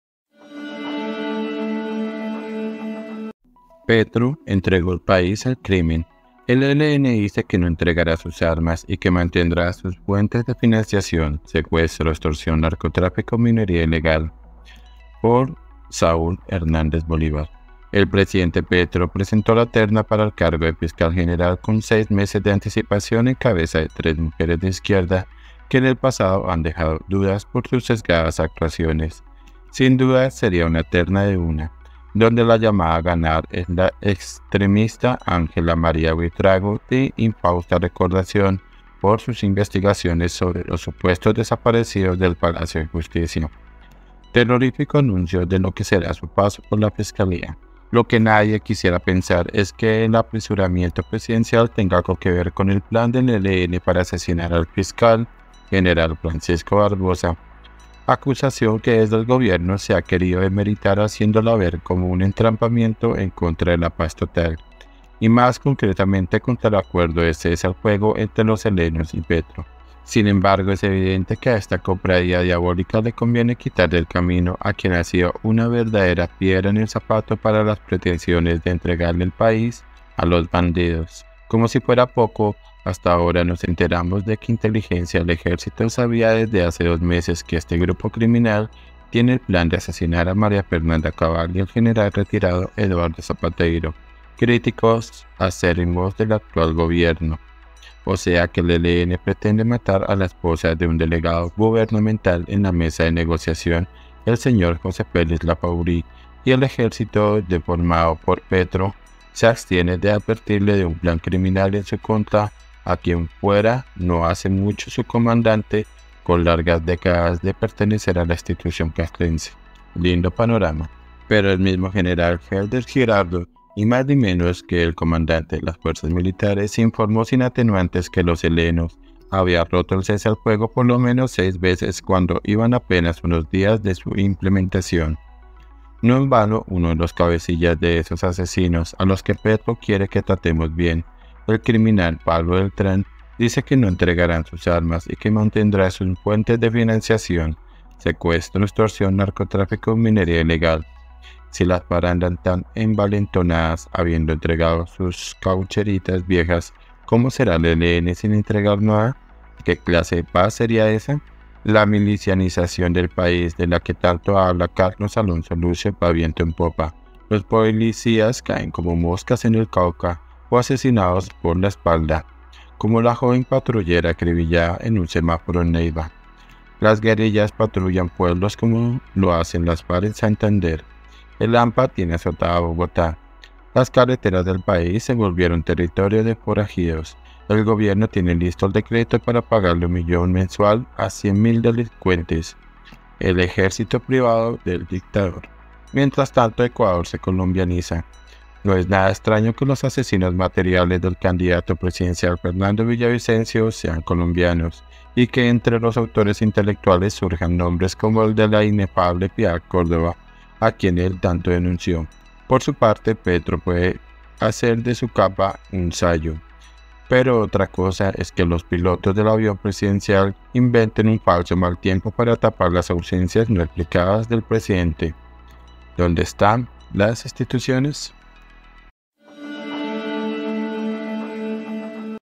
PETRO ENTREGÓ EL PAÍS AL CRIMEN ⛔ por Saúl Hernández Bolívar
El presidente Petro presentó la terna para el cargo de Fiscal General con seis meses de anticipación en cabeza de tres mujeres de izquierda que en el pasado han dejado dudas por sus sesgadas actuaciones.
columna SAÚL HERNÁNDEZ BOLIVAR